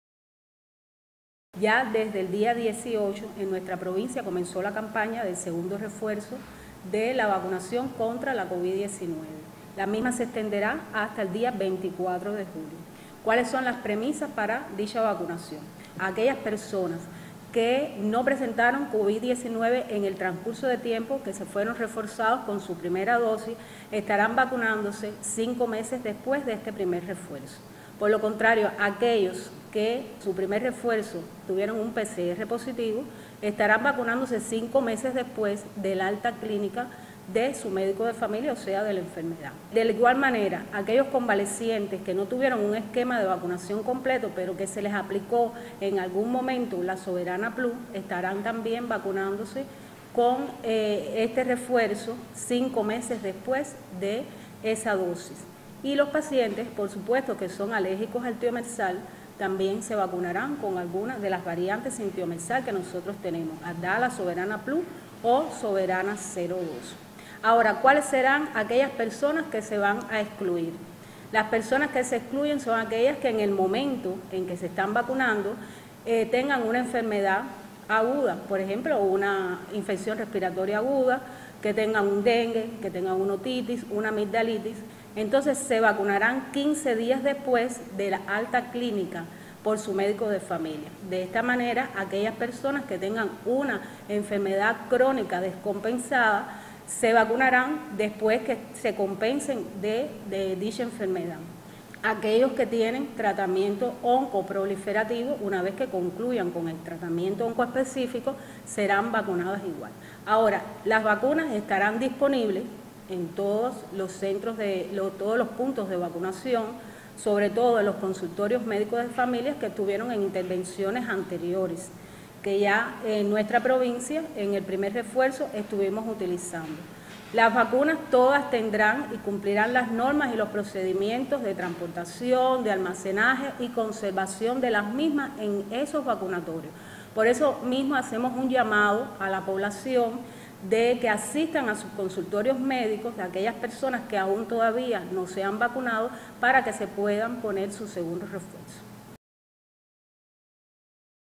Informa autoridad sanitaria sobre segundo refuerzo contra COVID-19.
En conferencia de prensa realizada en la Dirección Provincial de Salud, la Doctora Sadys Suárez del Villar Seuré, Jefa del Departamento de Enfermedades Transmisibles, explicó acerca del segundo refuerzo de la vacuna contra la COVID-19 en las edades comprendidas entre 19 a 49 años.